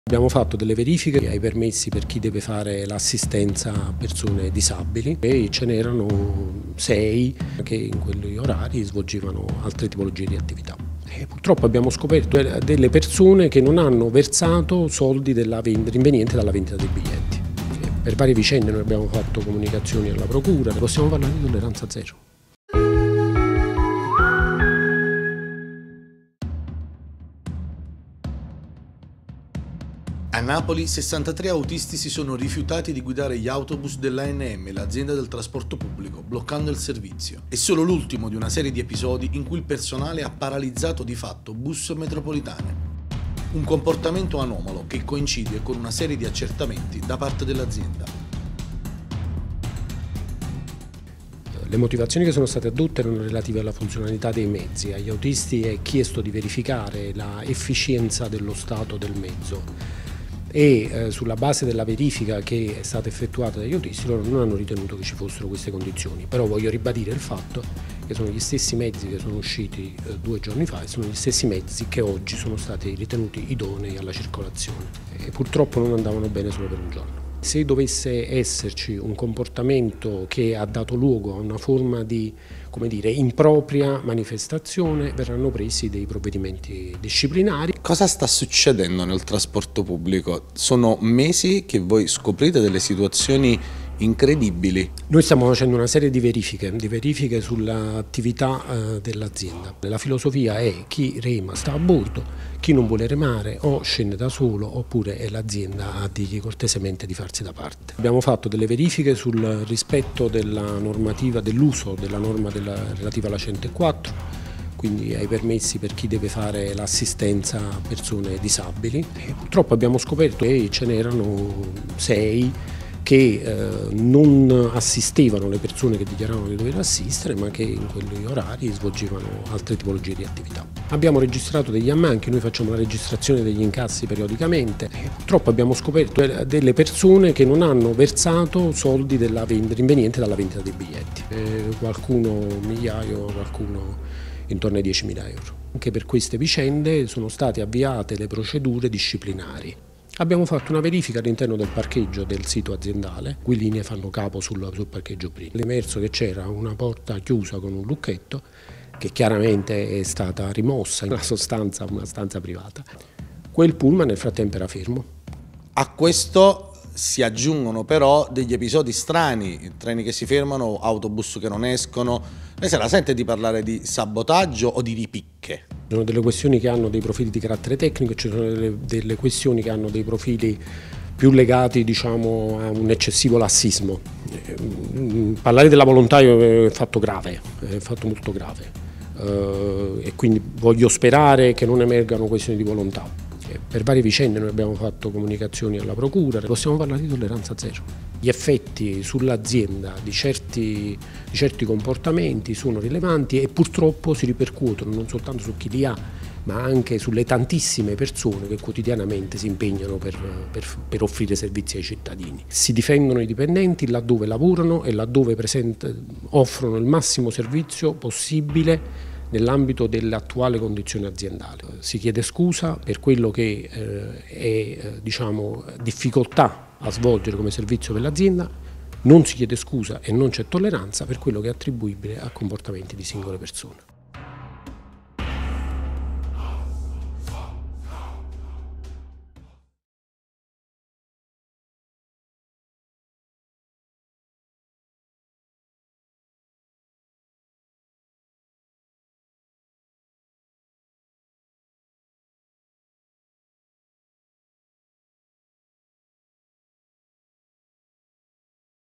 ANM tra furti, permessi truffa e bus fermi, Pascale: "Tolleranza zero, avvisati i giudici"
63 autisti si rifiutano di guidare gli autobus dell'ANM, perché considerati poco sicuri e paralizzano il servizio di trasporto pubblico, ma erano gli stessi che avevano guidato il giorno prima e il giorno dopo. E' caos tra i lavoratori dell'ANM per una serie di strane anomalie che coincidono però con una raffica di verifiche dell'azienda sui permessi, sugli incassi e sulle condizioni di lavoro. A Fanpage.it l'amministratore delegato di ANM, Nicola Pascale, spiega la situazione: "Abbiamo fatto verifiche su chi usufruisce dei permessi per assistere i disabili, in 6 svolgevano tutt'altra attività, abbiamo riscontrato degli ammanchi negli incassi dei biglietti, c'erano persone che sottraevano soldi tra i 1.000 e i 10.000 euro, nello stazionamento di parcheggio Brin una stanza era stata chiusa e usata privatamente mentre i bus erano fermi. Abbiamo consegnato tutto in Procura, avremo tolleranza zero".